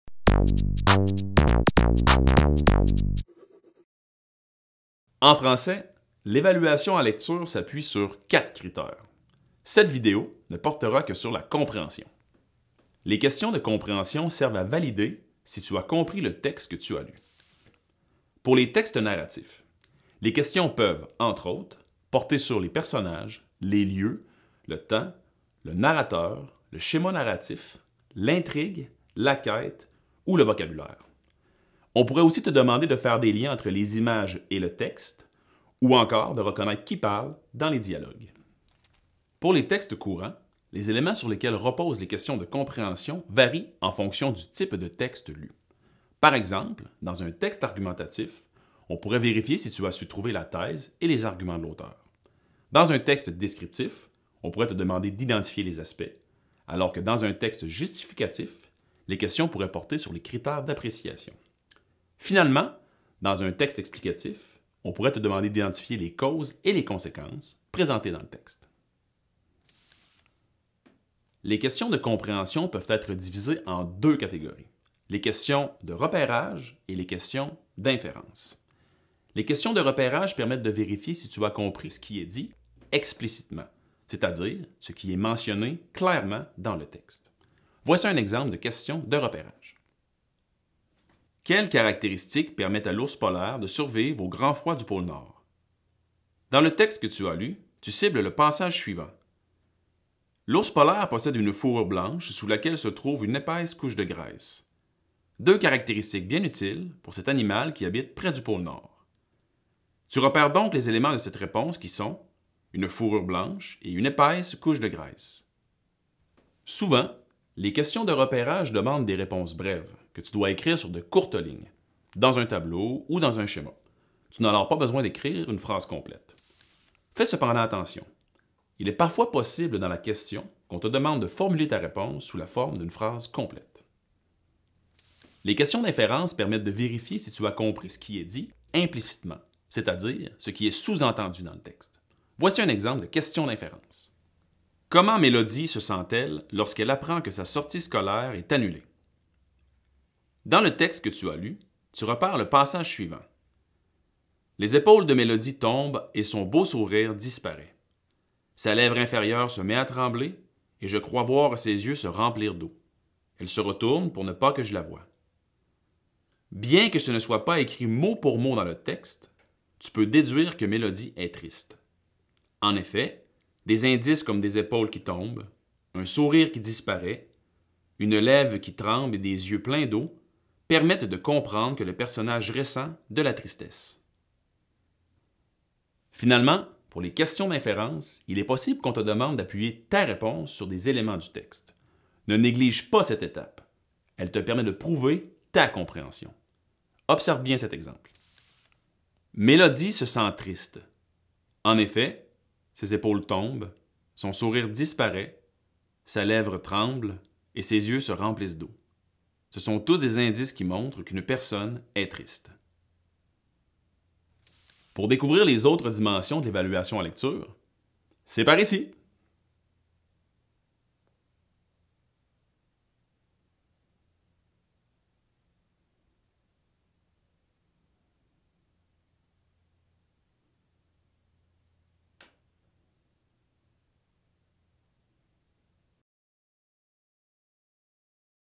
L'évaluation de lecture : la compréhension | Français | Alloprof
Cette vidéo t'expliquera comment répondre à des questions de compréhension (repérage et inférence) lors d'une évaluation de lecture.